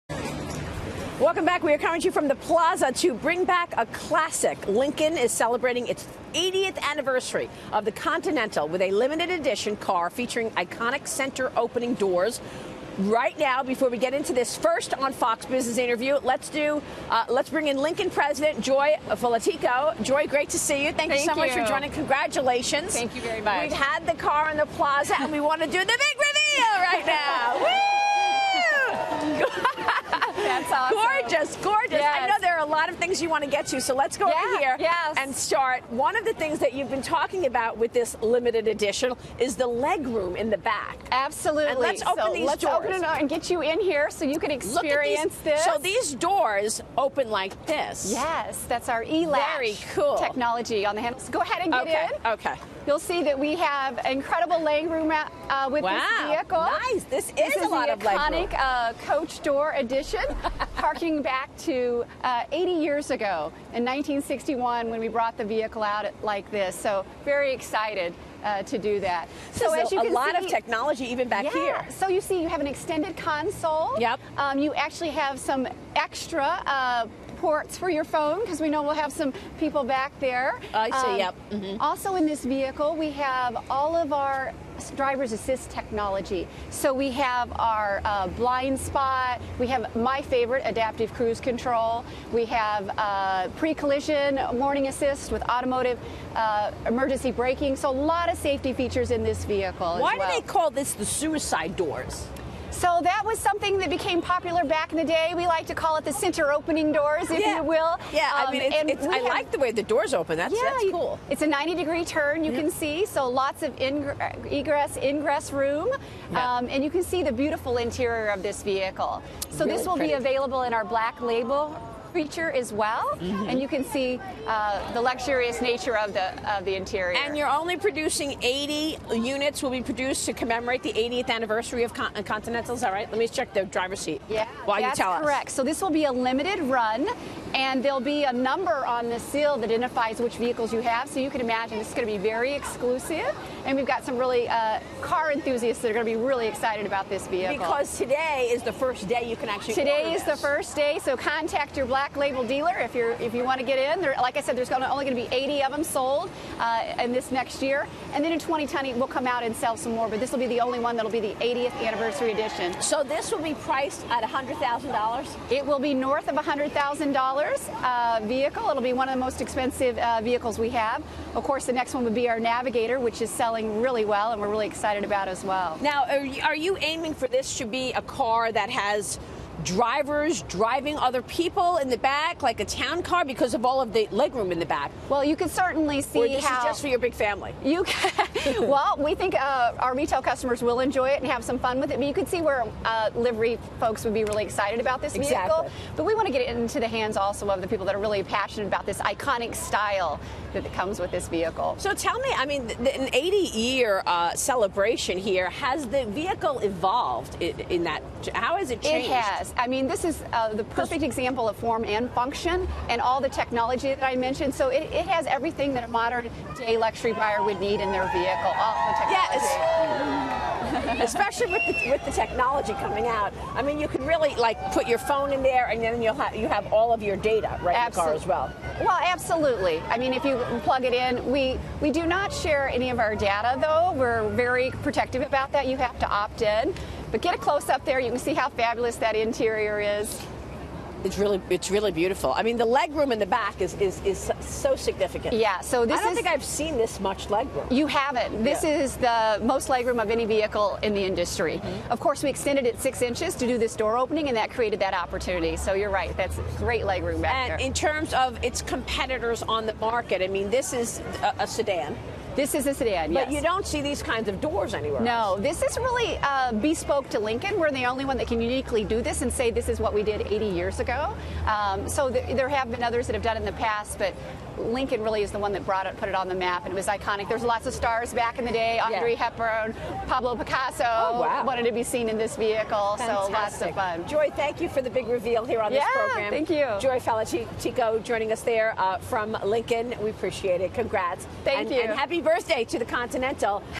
Lincoln celebrating 80th anniversary of the Continental
Lincoln President Joy Falotico on the 80th Anniversary edition of the Lincoln Continental.